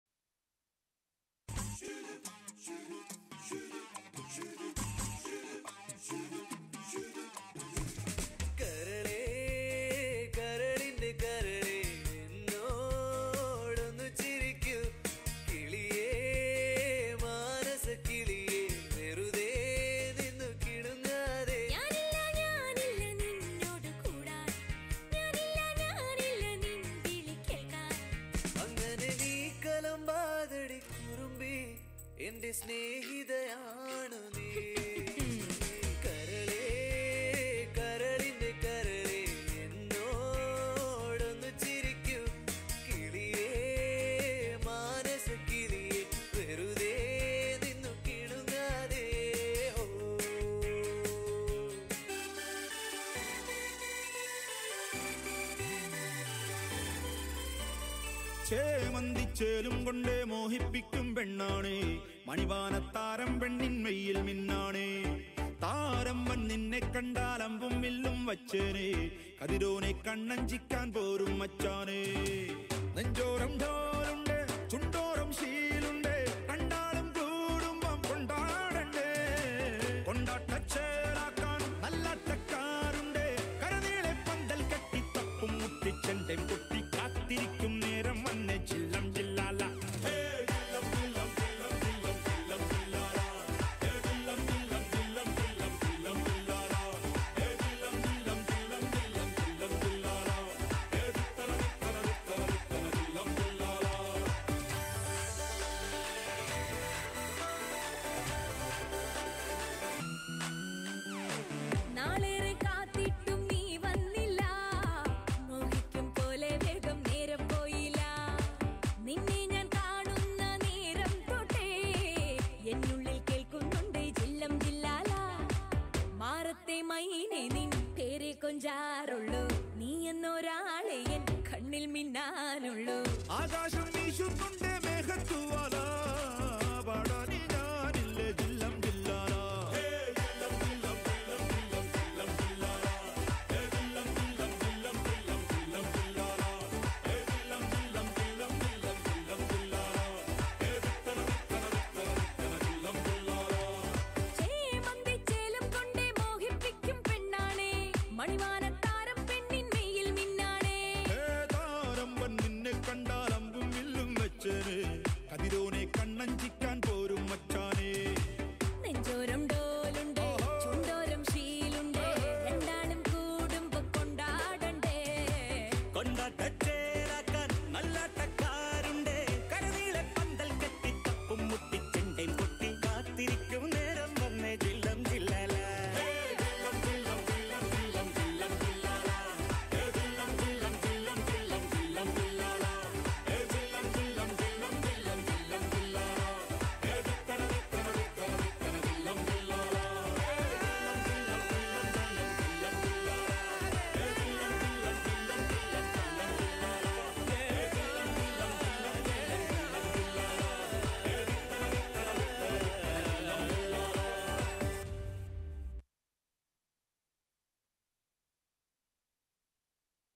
KCSMW Utsav Onam 2023 - Noopura Kids
Choreographer/Lead : Meera Cheviry / Archana Nair

Participants: Aaradhya Menon, Alvin Renju, Anushka Dipu Kumar, Devananda Priyesh, Ishika Sachin, Jane Manu, Shankar Sreejith, Sadhika Cheviry, Shreya Sreejith, Shraddha Vinod Nair, Vasudev Warrier, Vihaan Vineeth